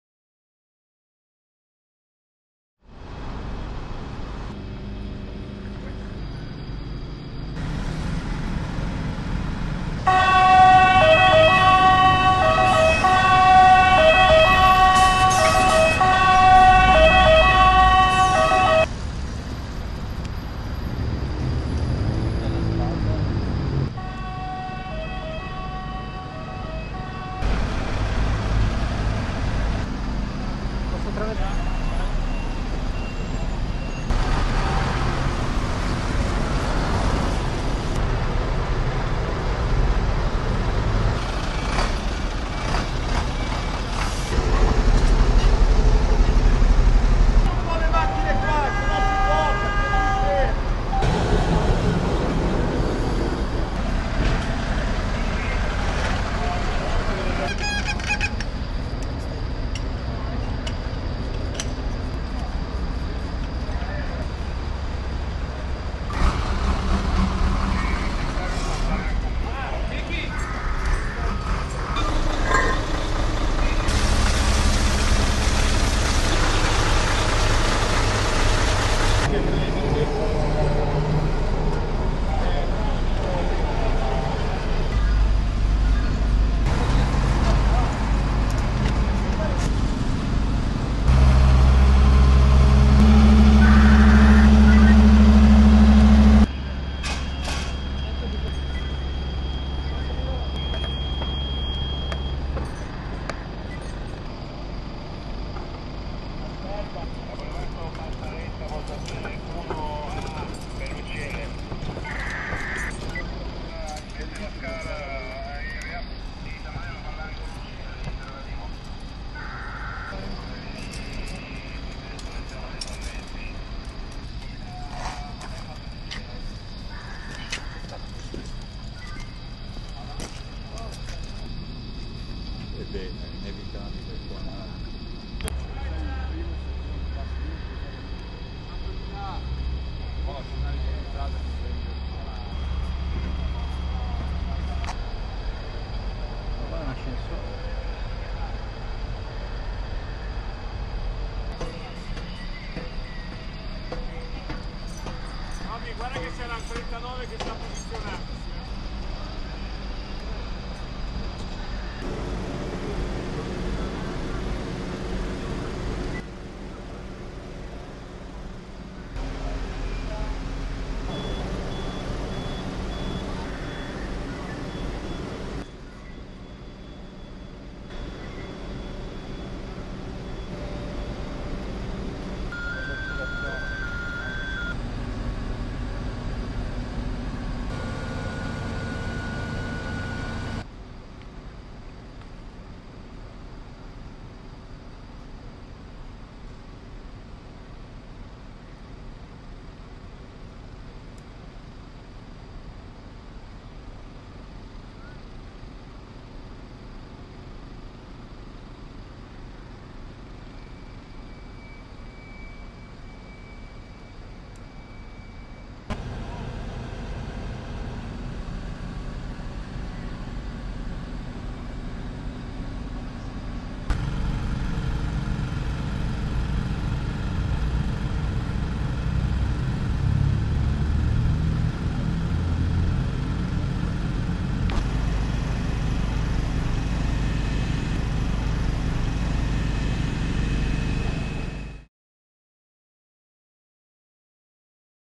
Esercitazione Vigili del fuoco 4 dicembre alla Scala
Questa sera i Vigili del Fuoco hanno eseguito una esercitazione antincendio al Teatro alla Scala di Milano.